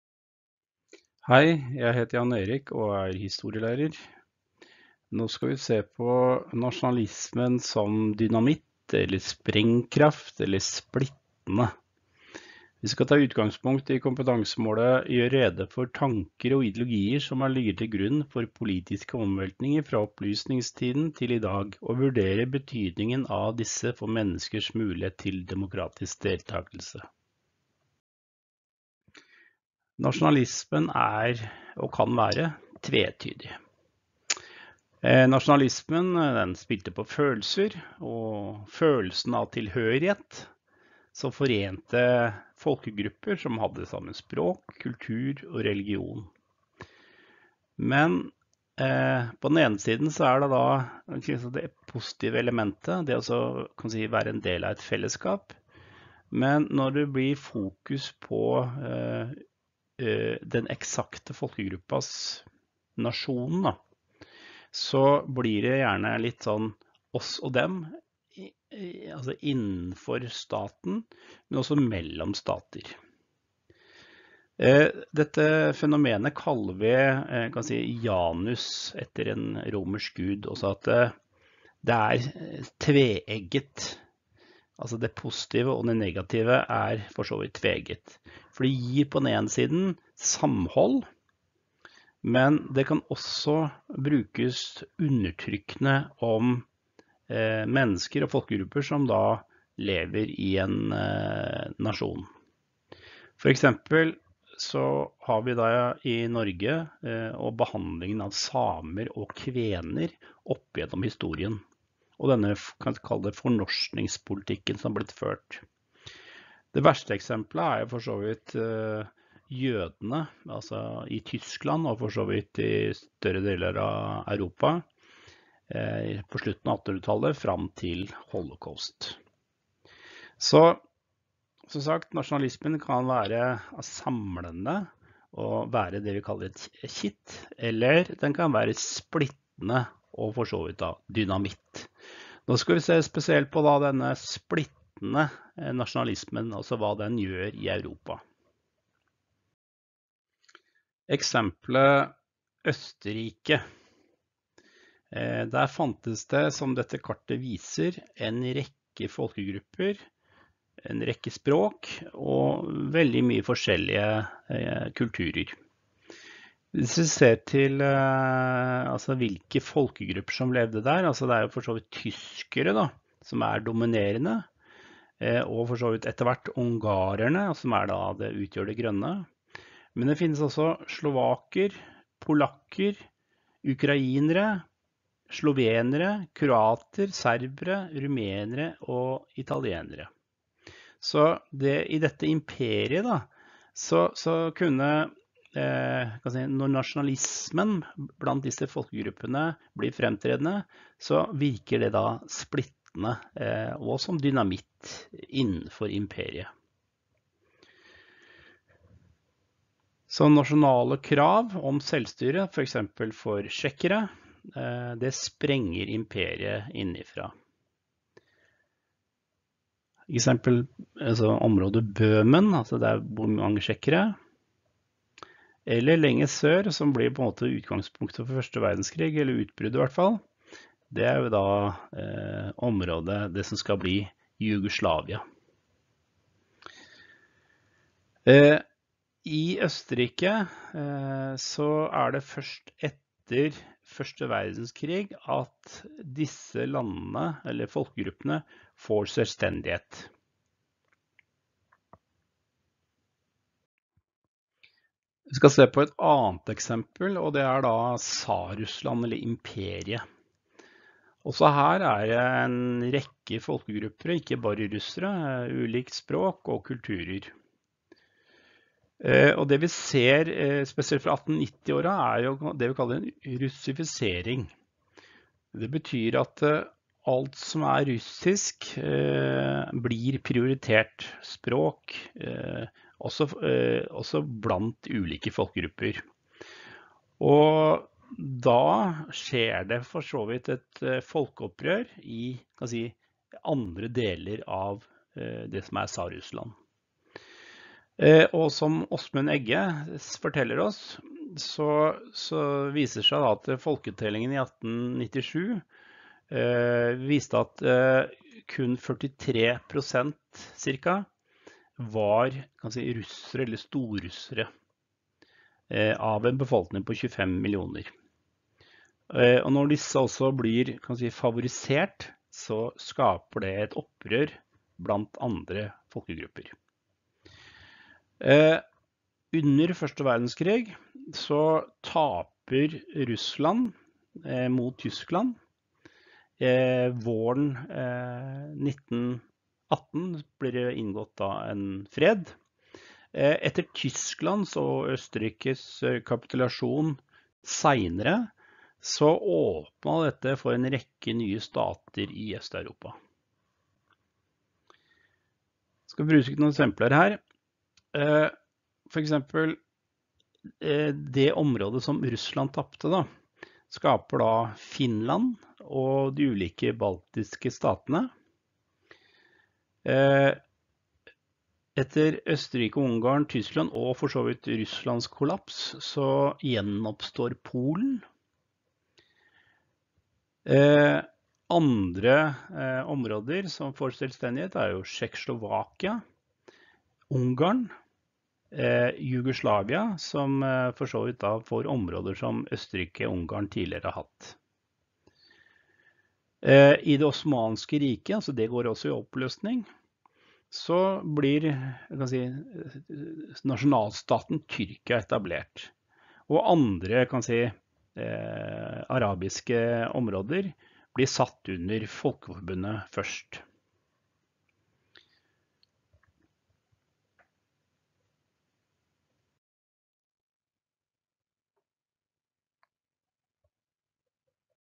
Historie: Nasjonalisme som dynamitt
Kompetansemål: HIS01-03: gjøre rede for tanker og ideologier som har ligget til grunn for politiske omveltninger fra opplysningstiden til i dag og vurdere betydningen av disse for menneskers muligheter til demokratisk deltakelse

00:00 Nasjonalismene som splittende
00:28 Kitt og dynamitt
02:30 Dynamitt i Østerrike
04:15 Splittelse i Tsar-Russland
05:54 Nye nasjonalstater i Øst-Europa
07:00 Kilder